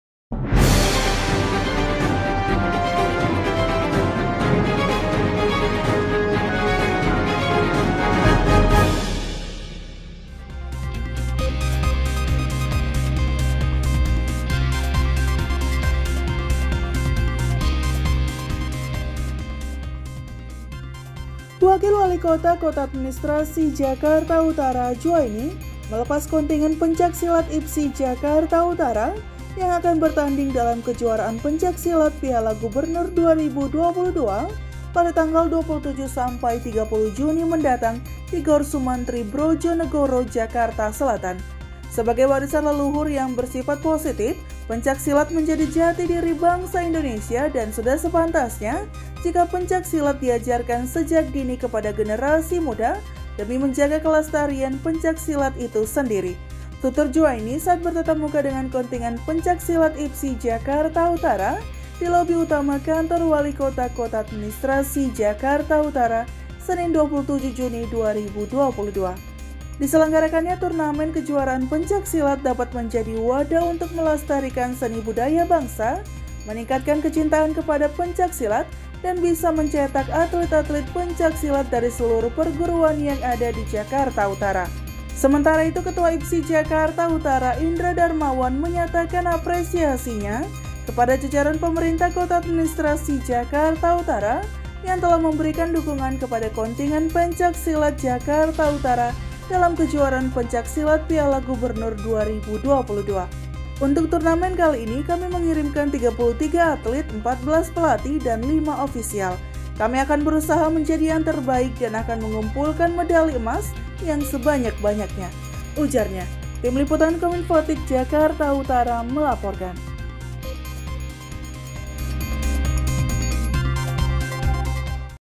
33 Atlit Pencak Silat Jakut Ikut Kejuaraan Pencak Silat Piala Gubernur 2022
KOMINFOTIK JU - Wakil Walikota Kota Administrasi Jakarta Utara, Juaini melepas kontingen pencak silat IPSI Jakarta Utara yang akan bertanding dalam Kejuaraan Pencak Silat Piala Gubernur 2022 pada tanggal 27-30 Juni mendatang di GOR Soemantri Brodjonegoro, Jakarta Selatan.

"Sebagai warisan leluhur yang bersifat positif, pencak silat menjadi jati diri bangsa Indonesia dan sudah sepantasnya jika pencak silat diajarkan sejak dini kepada generasi muda demi menjaga kelestarian pencak silat itu sendiri," tutur Juaini saat bertatap muka dengan kontingen pencak silat IPSI Jakarta Utara di Lobby Utama Kantor Walikota Kota Administrasi Jakarta Utara, Senin (27/6).

Diselenggarakannya turnamen kejuaraan pencak silat dapat menjadi wadah untuk melestarikan seni budaya bangsa, meningkatkan kecintaan kepada pencak silat, dan bisa mencetak atlit-atlit pencak silat dari seluruh perguruan yang ada di Jakarta Utara.

"Turnamen ini juga untuk mendorong pencarian atlit-atlit muda yang berbakat agar bisa bermunculan. Dengan banyaknya atlit yang menunjukan bakat dan potensinya maka saya berharap agar semua perguruan pencak silat, IPSI, dan juga KONI bisa terus mengembangkan dan meningkatkan kualitas para atlit untuk bisa meraih prestasi," ungkap Juaini didampingi Asisten Administrasi dan Kesejahteraan Rakyat Sekko Administrasi Jakarta Utara, Muhammad Andri, Kasudin Pemuda dan Olahraga Kota Administrasi Jakarta Utara, Heru Haryanto.

Sementara itu, Ketua IPSI Jakarta Utara, Indra Darmawan menyatakan apresiasinya kepada jajaran Pemerintah Kota Administrasi Jakarta Utara yang telah memberikan dukungan kepada kontingen pencak silat Jakarta Utara dalam Kejuaraan Pencak Silat Piala Gubernur 2022. "Untuk turnamen kali ini, kami mengirimkan 33 atlit, 14 pelatih, dan 5 official. Kami akan berusaha menjadi yang terbaik dan akan mengumpulkan medali emas yang sebanyak-banyaknya," ujarnya.

Meraih Piala Gubernur 2022 menjadi impian para atlit pencak silat di DKI Jakarta. Salah satunya adalah Jovanda Maria (18), atlit pencak silat dari Kecamatan Tanjung Priok. "Saya tertarik untuk berlatih pencak silat sejak kelas 1 SMP karena pencak silat itu keren dan lebih menantang. Mudah-mudahan di Kejuaraan Piala Gubernur 2022, saya bisa memberikan hasil yang terbaik untuk Jakarta Utara dan bisa melaju ke PON," harap Jovanda yang akan bermain di kelas pertandingan E Putri Dewasa.